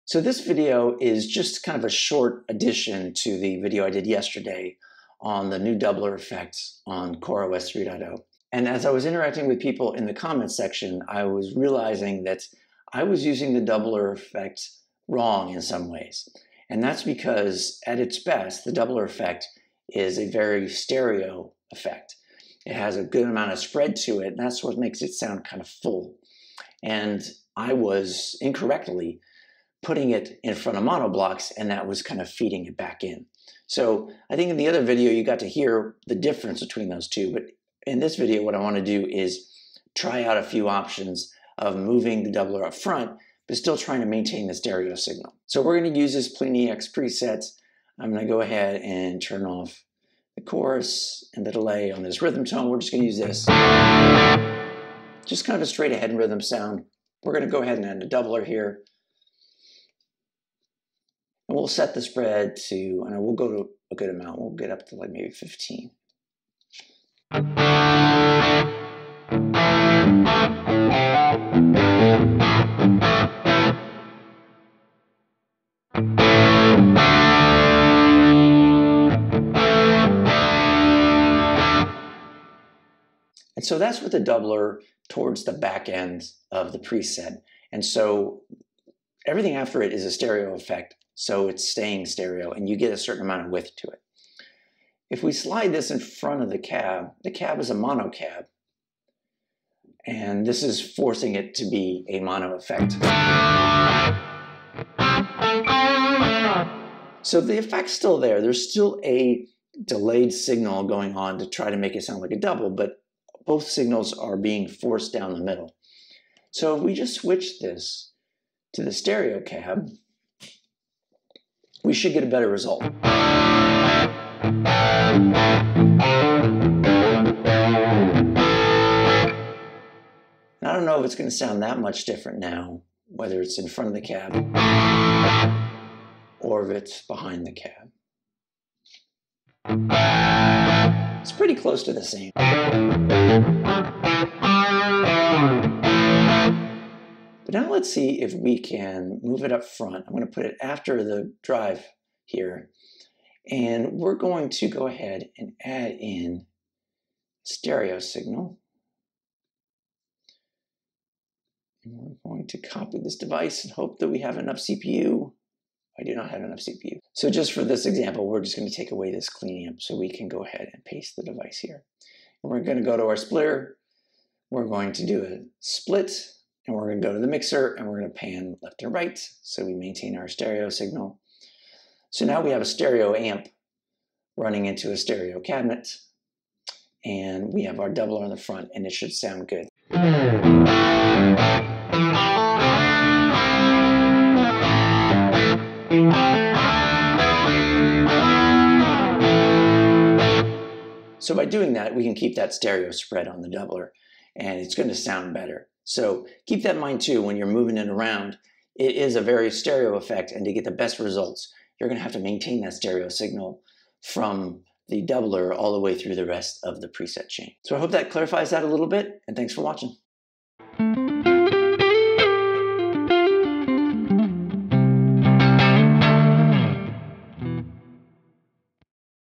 I Used the CorOS 3.0 Doubler Wrong
After messaging with some folks in the comment section of my last video, I came to the conclusion that I was using the Doubler from CorOS 3.0 wrong. In this video we'll look how to get a better result.

You can find the preset and captures under my Neural account (davlavMusic).